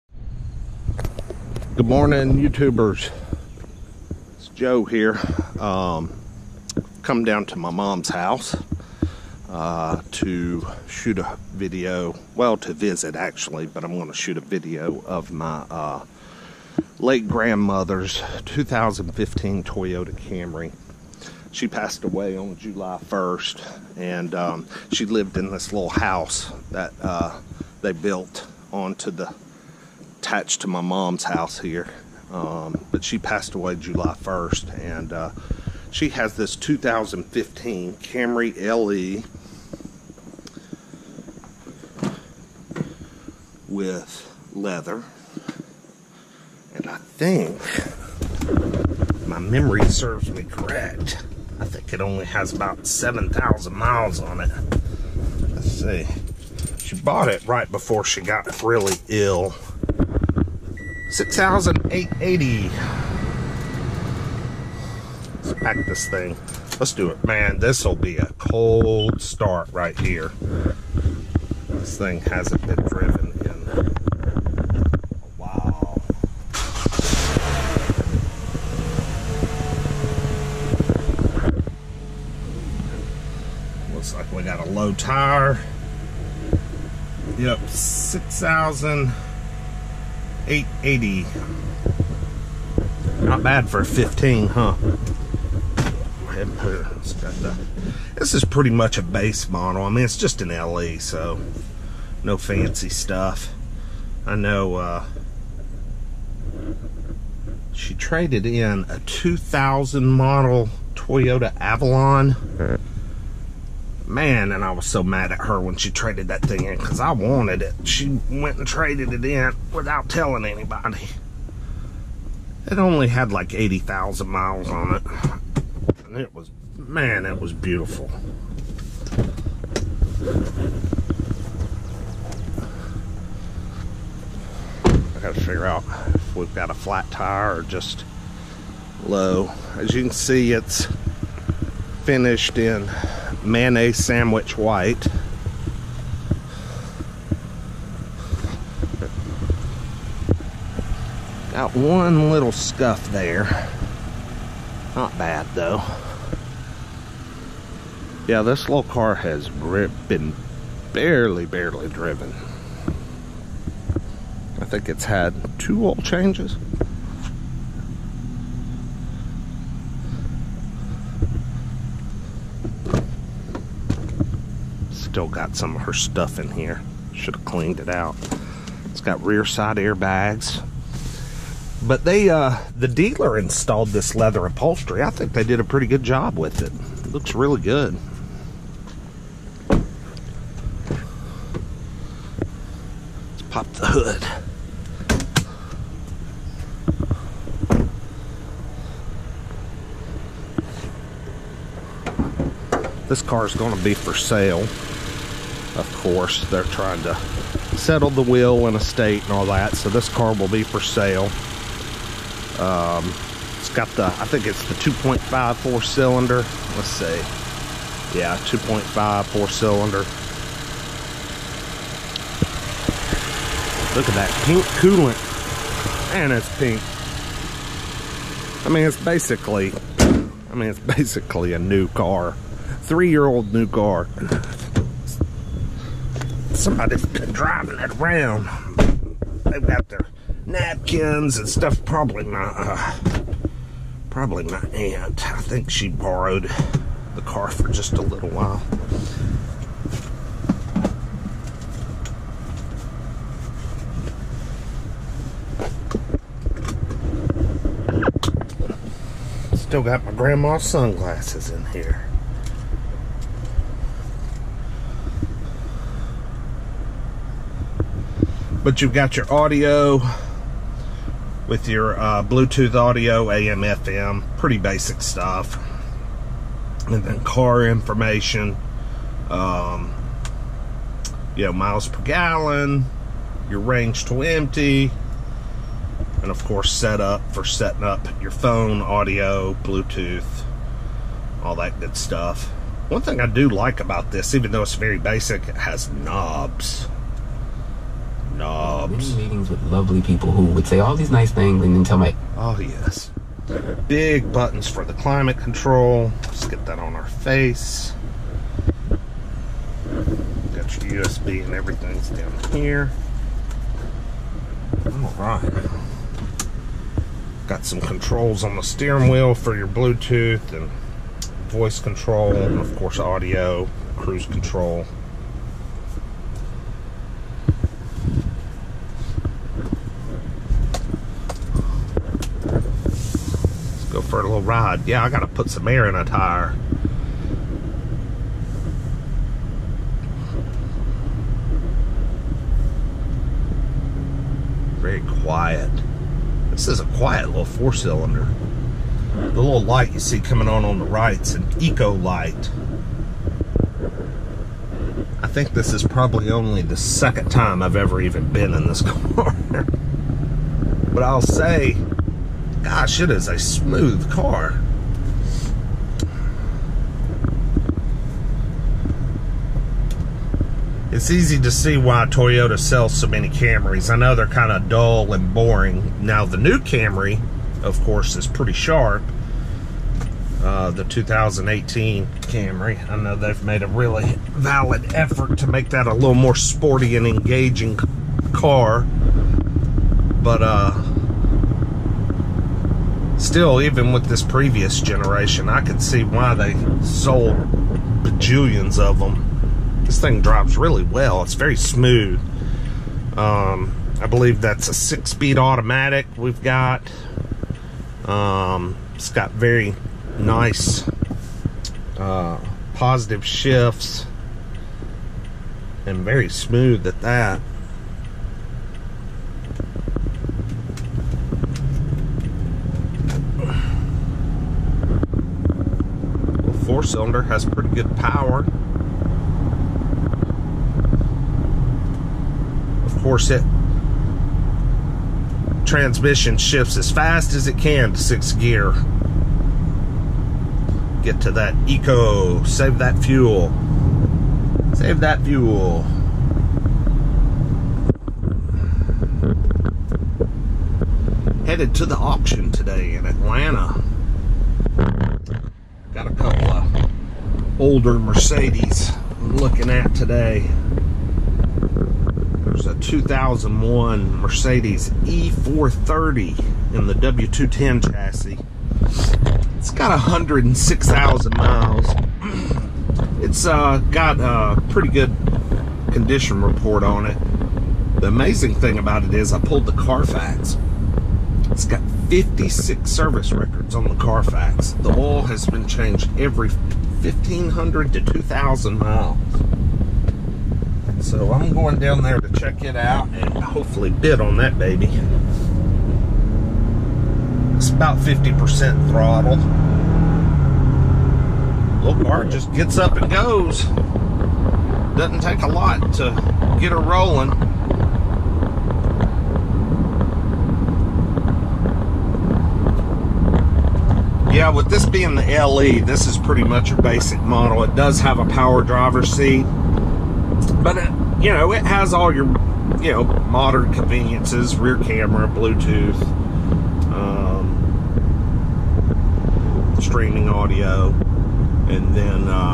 2015 2016 2017 Toyota Camry LE 4 cyl 2.5L Test drive 0-60 pov review (PT 1)
This is my grandmas 2015 Camry. She purchased it new in 2015 and it only has 6800 miles on it. My grandma just passed a few weeks back and her estate is being settled car will be for sale soon!

THIS IS PART 1   PART 2 TO FOLLOW   THIS NEW CAMERA IS PLAYING TRICKS ON ME ...ARG STILL TRYING TO LEARN